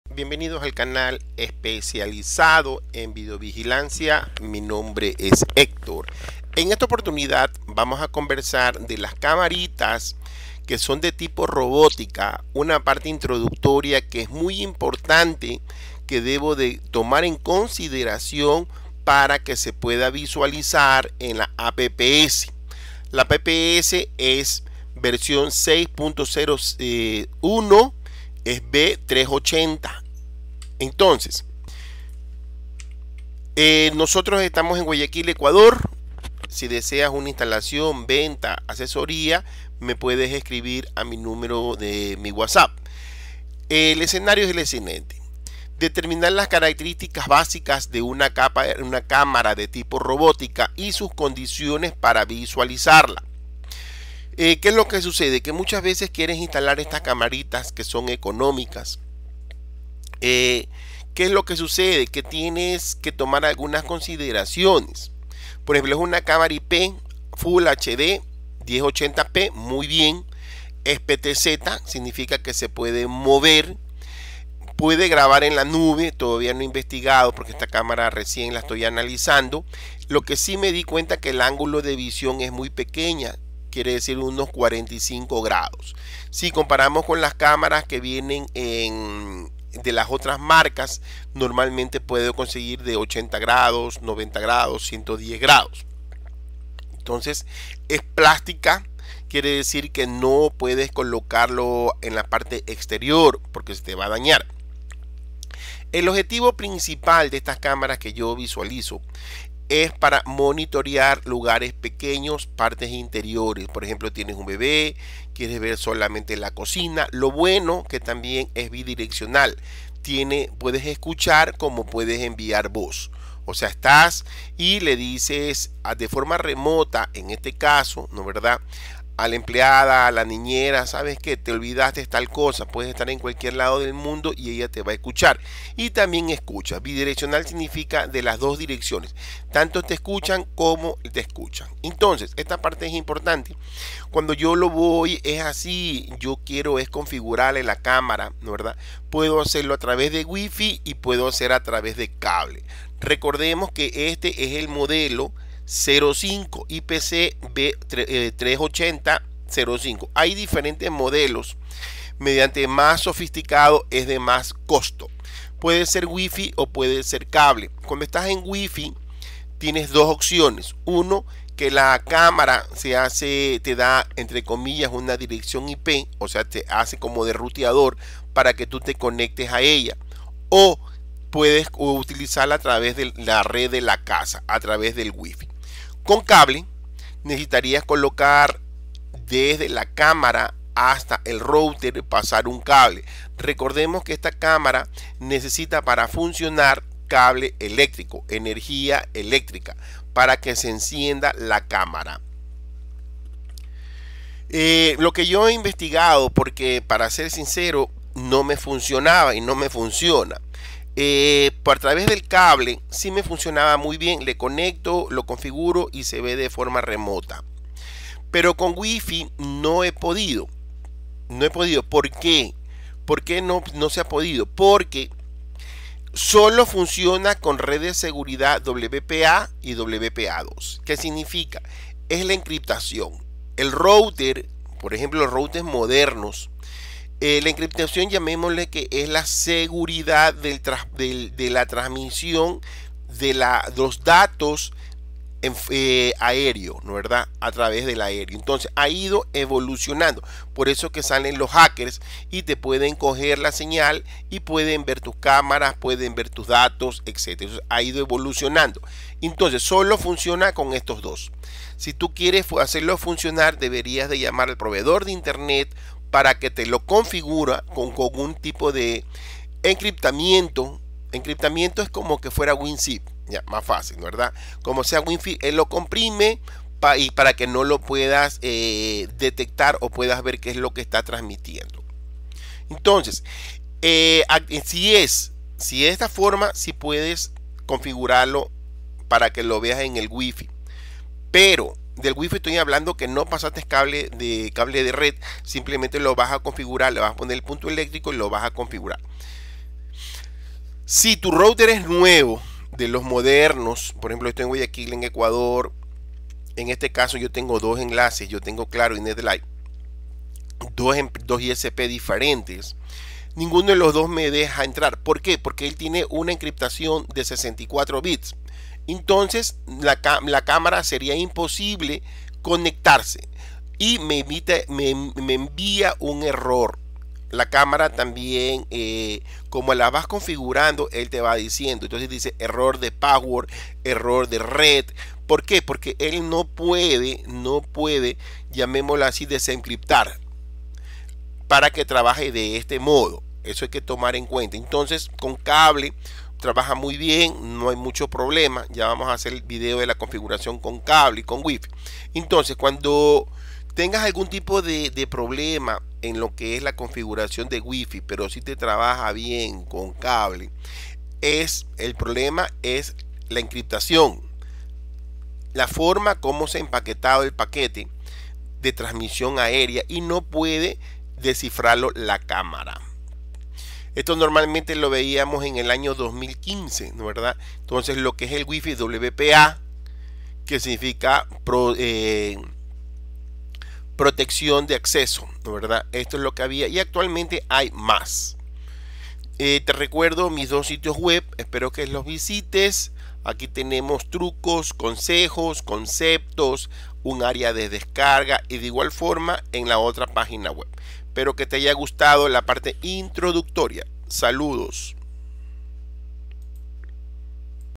Introducción Cámara Rebotica V380-Q5
Introducción y consideraciones al configurar una cámara WiFi Smart IPC-V380-Q5

Guayaquil - Ecuador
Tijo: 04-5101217

Guayaquil - Ecuador